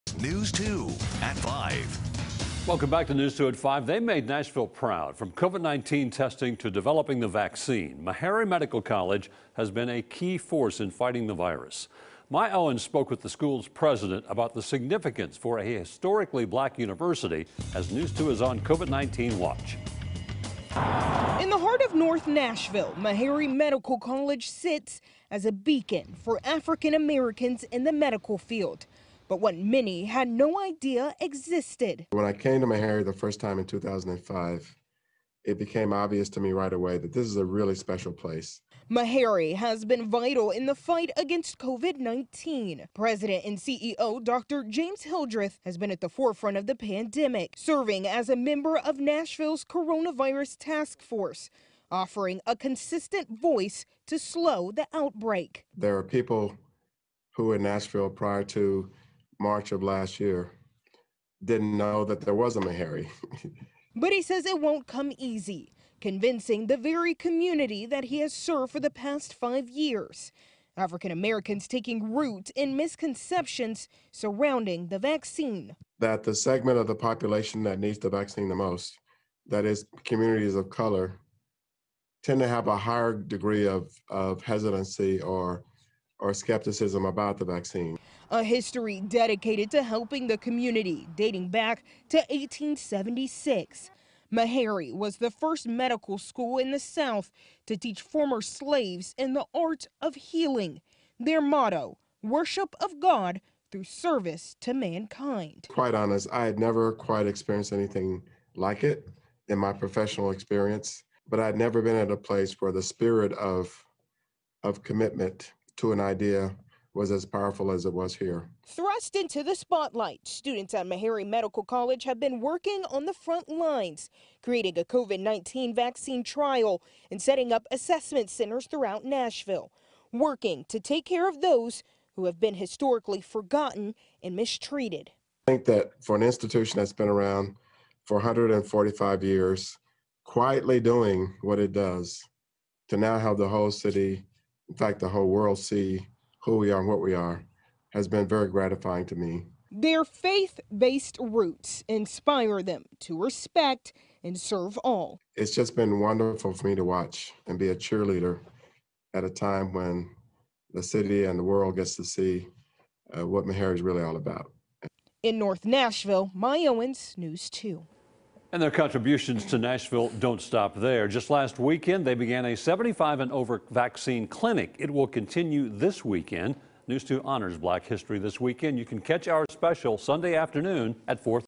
MeHarry on the frontlines
MeHarry Medical College is on the frontlines of vaccine, testing, research and development during the COVID-19 pandemic.